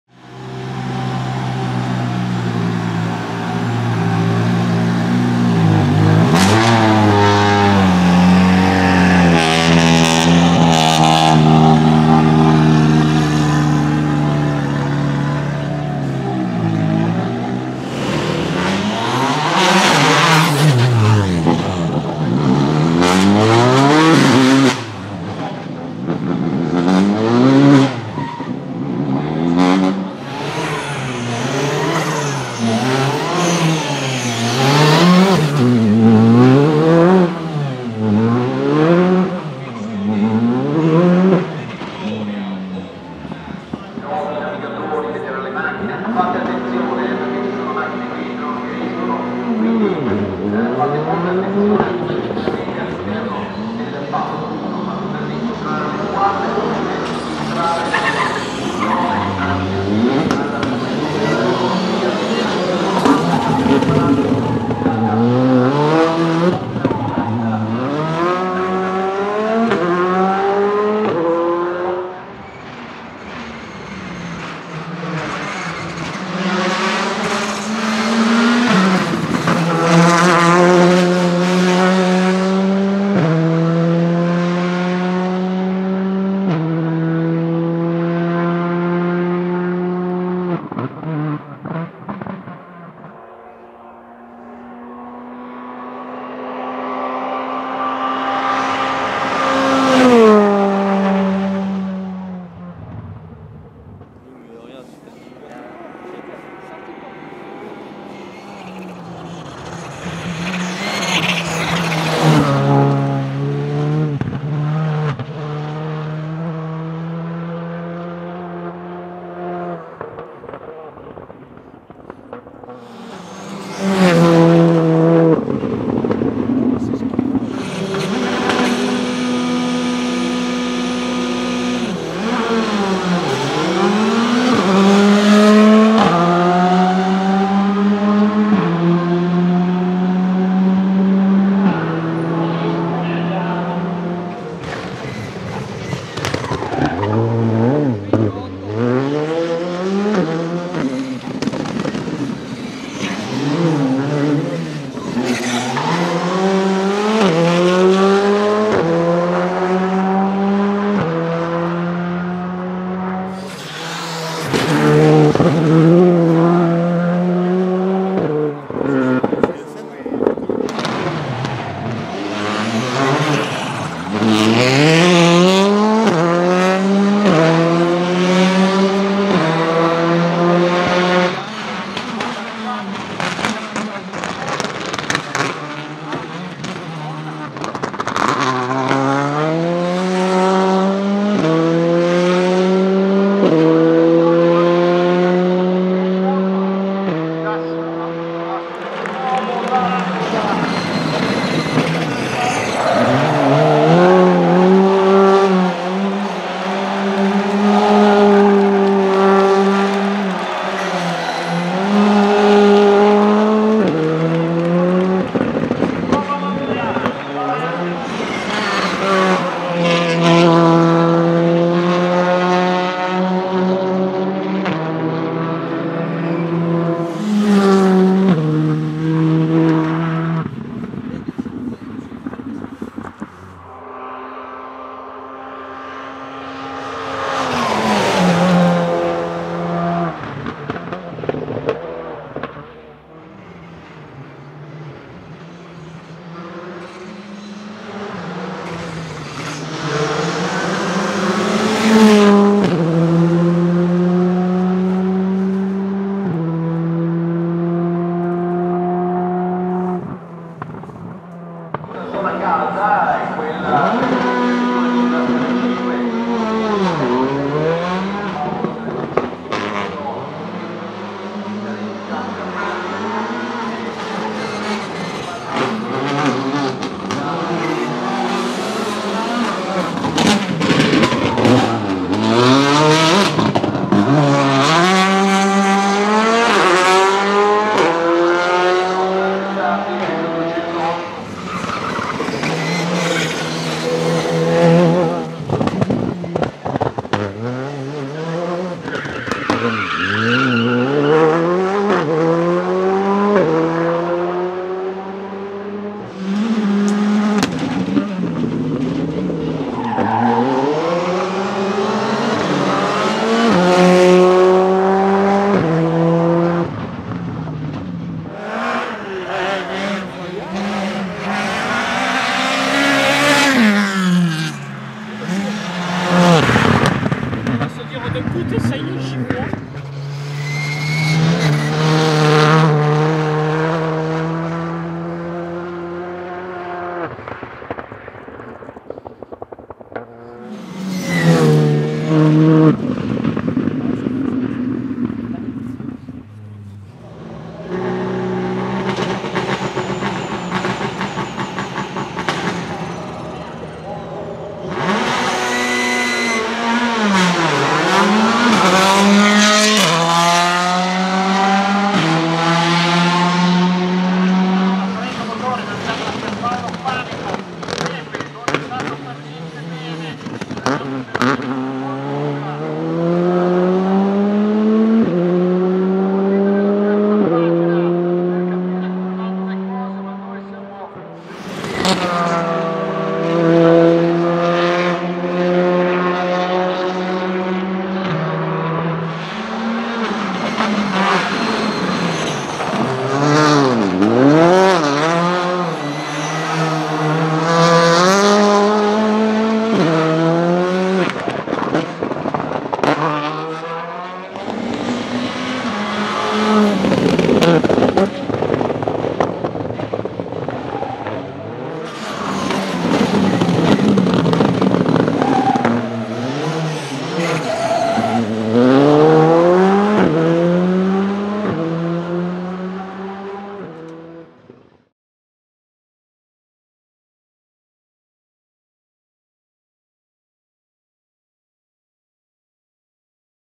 Škoda Fabia R5 Rally Car Sound - Starts, Accelerations, Anti-Lag & More
Watch and hear various Škoda Fabia R5 in action during some events I was in the last few months!

Camcorder: Canon Legria HF G40 + Canon DM-100 Microphone
Events: Monza Rally Show 2016, Vedovati Ronde Monza 2016 & WRC Rallye Monte Carlo 2017
Where: Monza, Italy / Gap, France